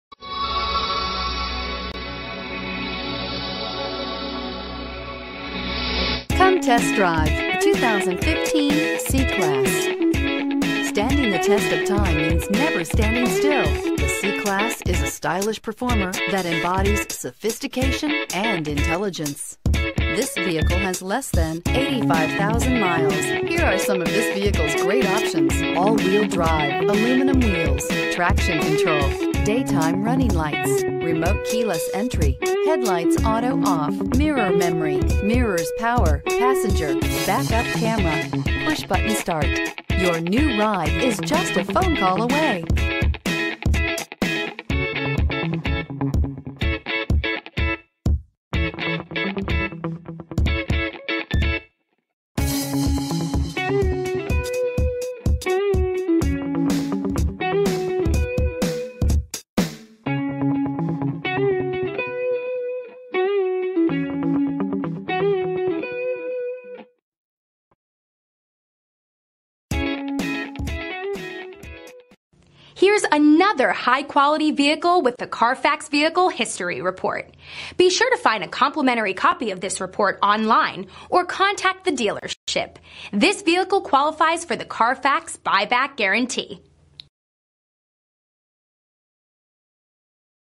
2015 Mercedes-Benz C-Class C 300 Minnetonka Minneapolis Wayzata,MN 27422A
Sears Imports
13500 Wayzata Blvd

18 AMG?? 5-Spoke Alloy Wheels, Burmester Premium Sound, Heated Front Seats, KEYLESS GO??, LED Headlamps, MB-Tex Upholstery, Panorama Sunroof, Premium 1 Package, SiriusXM Satellite Radio, Split folding rear seat, Sport Package. Recent Arrival! Clean CARFAX. 24/31 City/Highway MPGbrbrbr2015 Mercedes-Benz C-Class C 300 4MATIC??